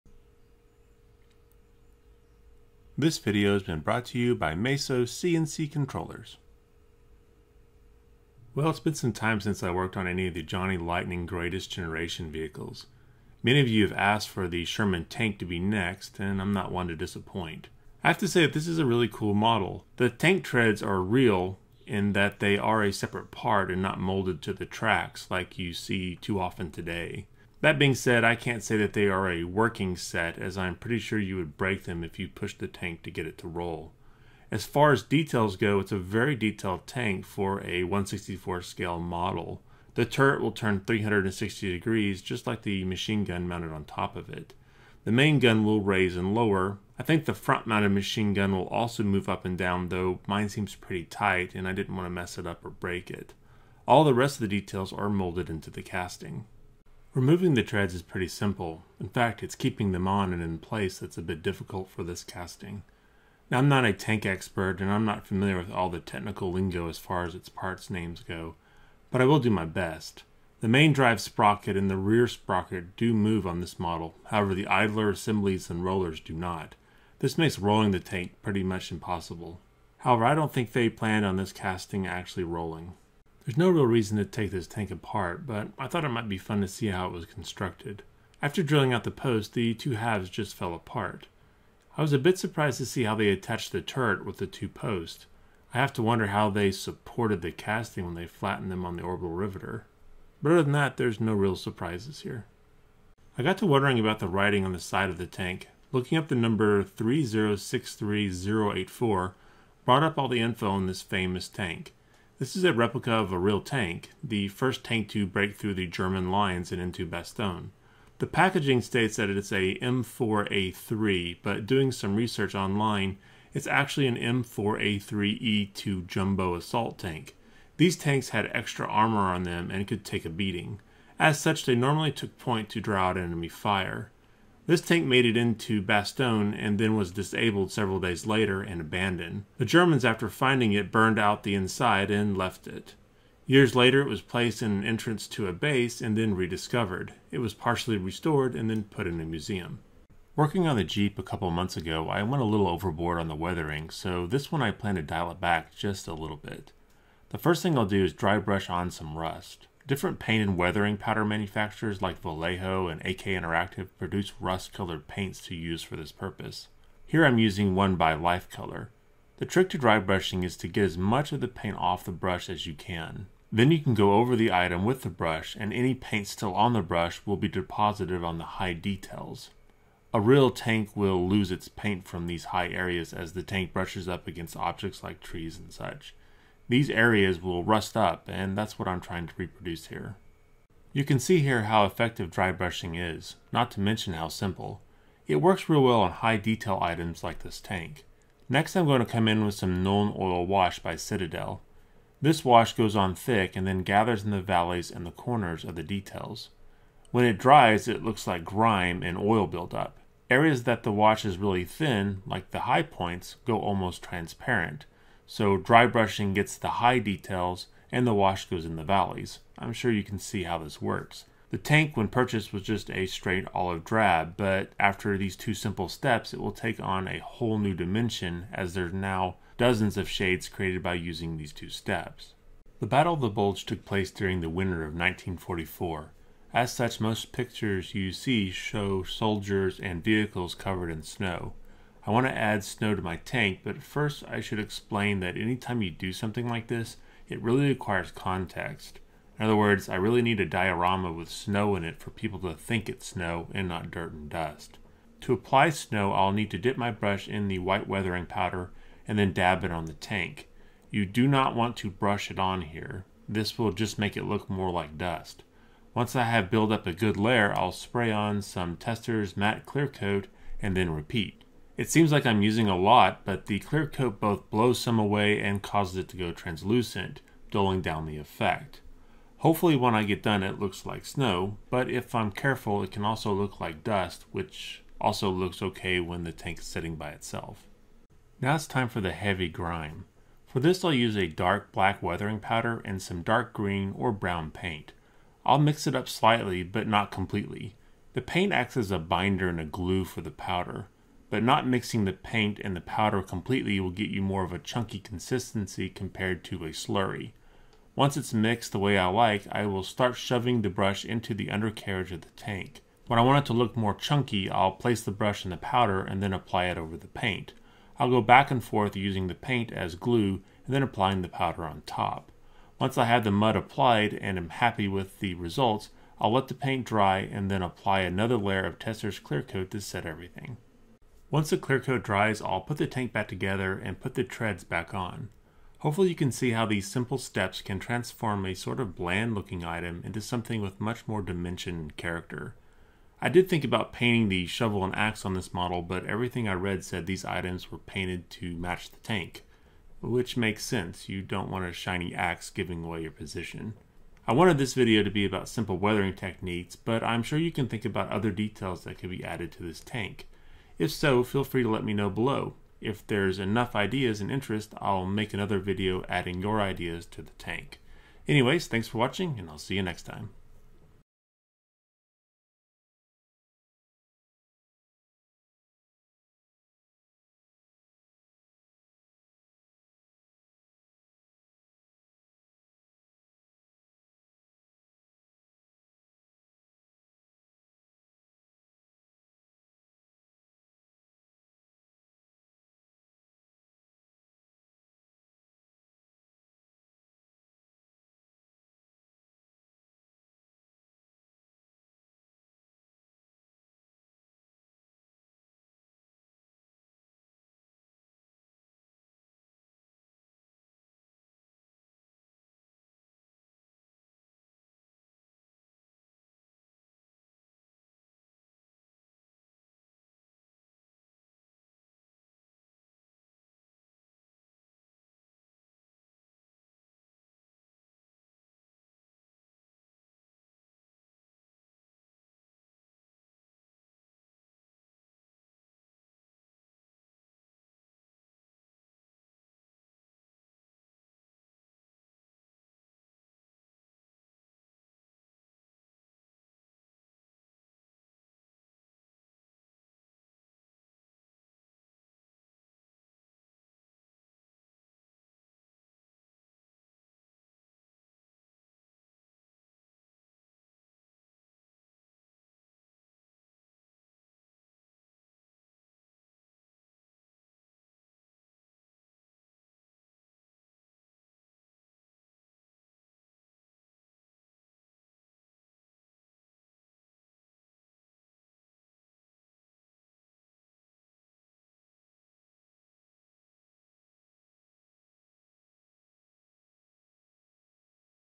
Simple and Easy Weathering Techniques on a 1:64 Sherman Tank
In this video I will show some quick and simple weathering techniques you can use to "spice" up your 1:64 scale vehicles.  This is the Johnny Lightning M4A3 Sherman Tank from the Greatest Generation line.